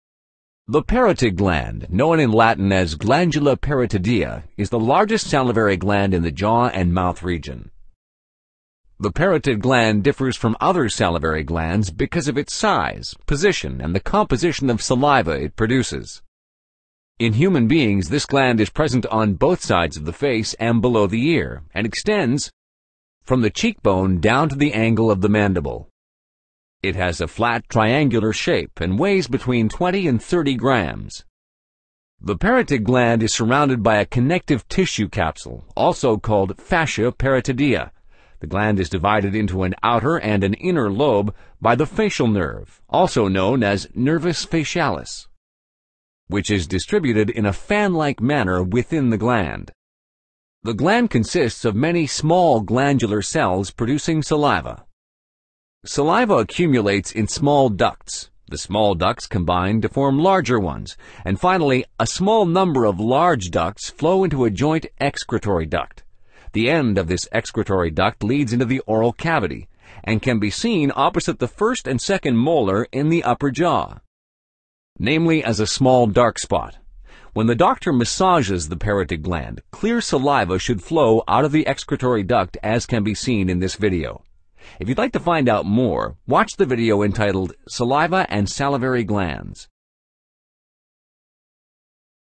What is the Parotid Gland?
The largest and thereby the most important salivary gland in the mouth is known as the Parotid gland or the glandula parotidea. The saliva produced from this gland is transported from gland though the excretory duct called the Parotid duct. This duct passes between the second upper molar teeth and reaches the cheeks. The clear saliva is even visible to the eye when the Parotid gland is gently massaged to stimulate secretion. What's more, the saliva acts as a cleansing agent with anti-bacterial properties. This saliva helps to cleanse any traces of inflammation or infections that occur in glands due to improper dental hygiene. People who have relatively lesser saliva secretion are more prone to gland inflammatory diseases as the pathogenic bacteria can reach the vulnerable glands very easily and cause infection. Watch this video to know more about the benefits of saliva secretion and the glands that produce it.